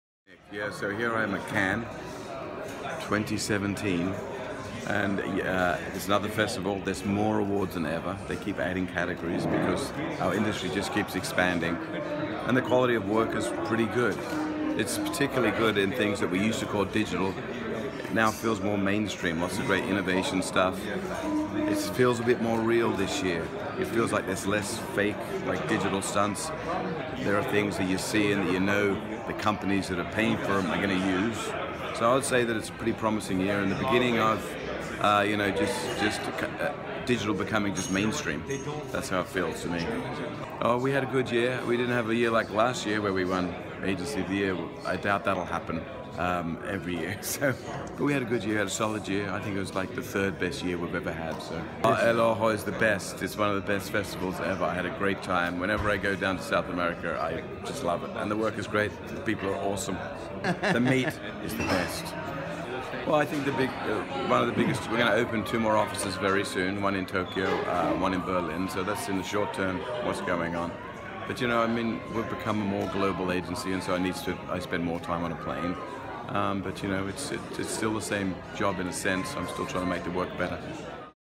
Nick Law
Nick Law, Chief Creative Officer de R/GA Worldwide, comparte su visión de Cannes Lions 2016 y cuenta cómo vivió la experiencia de participar en El Ojo de Iberoamérica 2015.